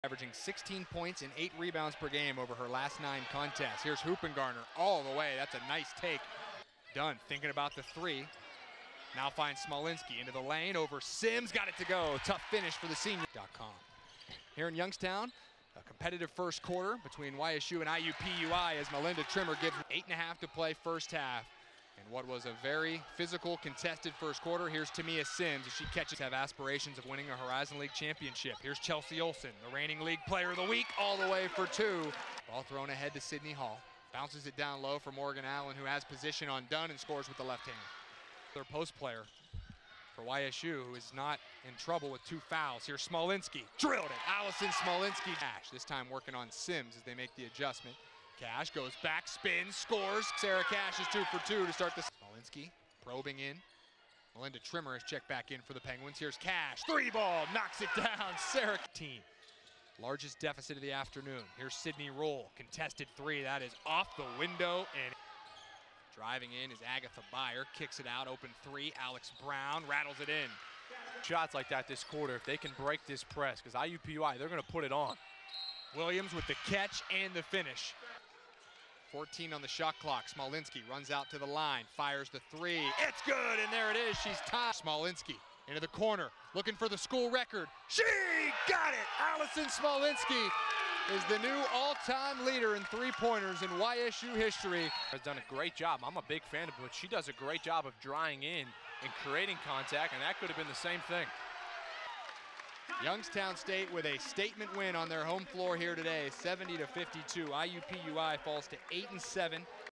YSU vs. IUPUI Women's Basketball Highlights | Jan. 5, 2019`
Highlights from the Youngstown State women's basketball team's 70-52 win over IUPUI on Jan. 5, 2019 at the Beeghly Center.